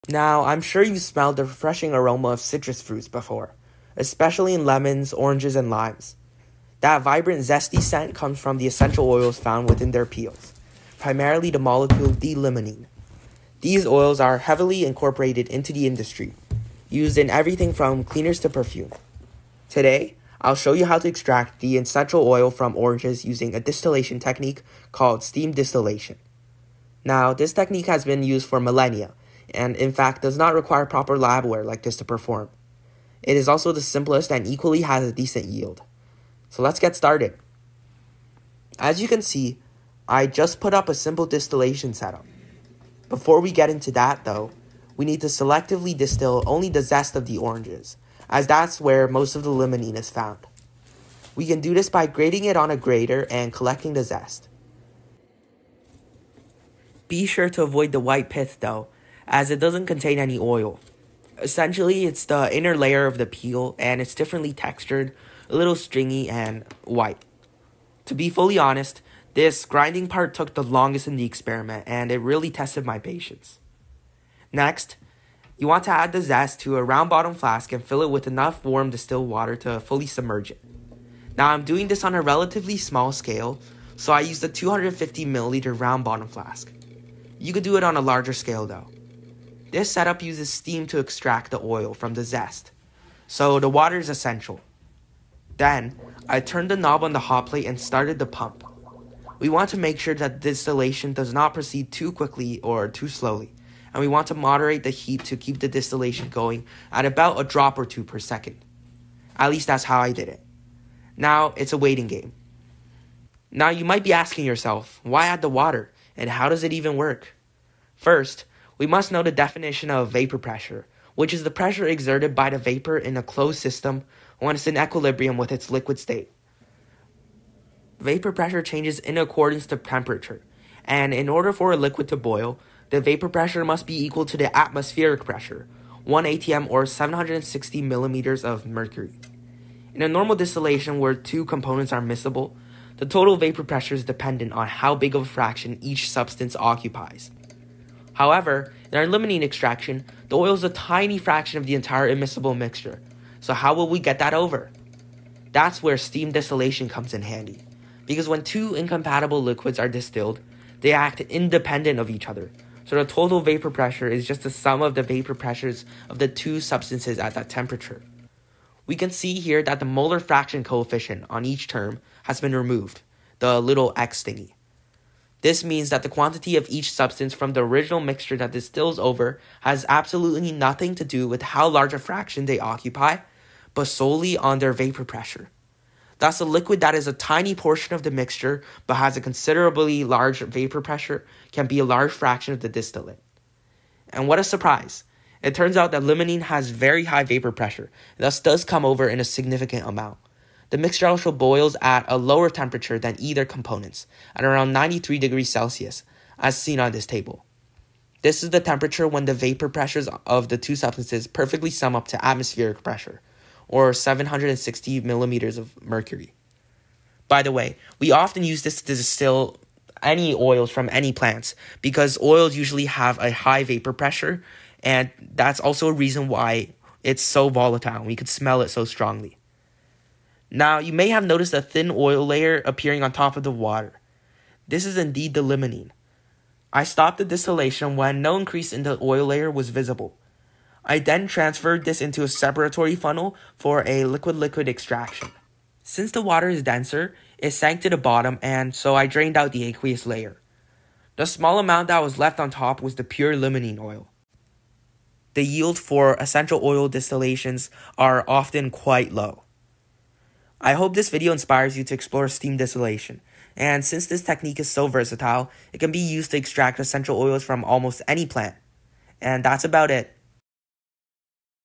Extracting the Essential Oil from Oranges
In this video, I extract the essential oil limonene from oranges, and this can be done through a variety of ways on different citrus fruits. I used steam distillation for the extraction of limonene.

Limonene, a fragrant compound found in citrus peels, can be extracted from various citrus fruits like lemons, oranges, and limes. Steam distillation is an efficient method that separates the essential oil from the fruit's peel, showcasing the fascinating chemistry behind essential oil extraction involved in the cleaners, perfumes and flavors in our everyday lives..

Whether you're a science enthusiast or just curious about the process, this experiment highlights a simple yet effective technique to isolate this versatile and aromatic compound.